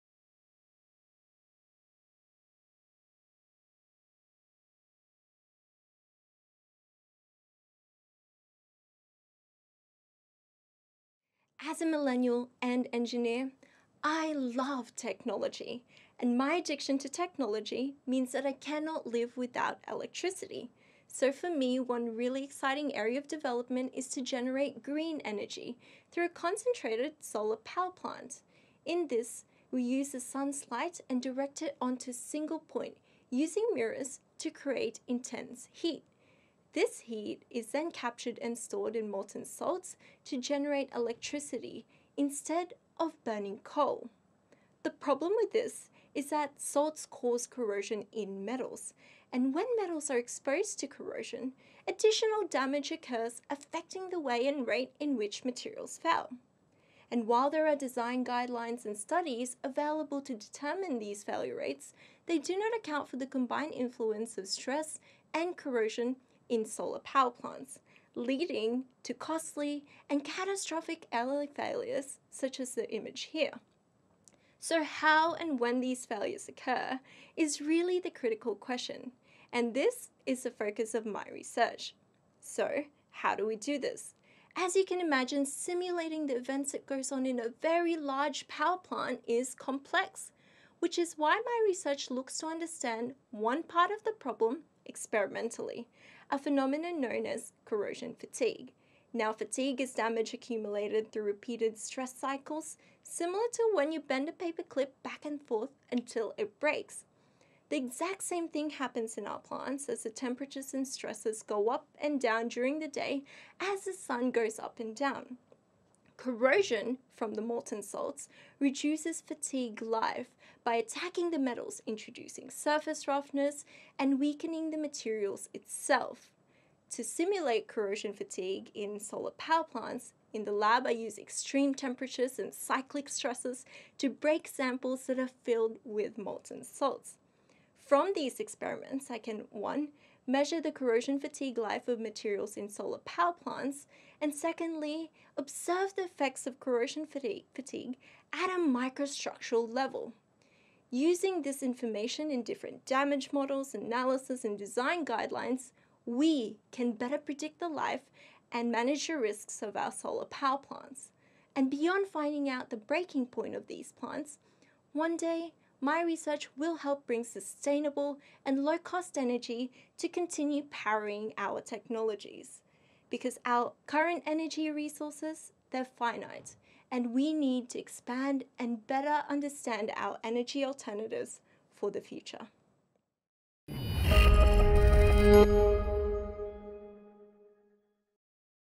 Three Minute Thesis (3MT) 2021 Finalist – Elaiza Luker, QUT Engineering Faculty
Elaiza Luker, from QUT’s Engineering Faculty was one of the 2021 QUT finalists. Her presentation was entitled, ‘The Breaking Point of Concentrated
Solar Power Plants’. For more information about research education and training offered at QUT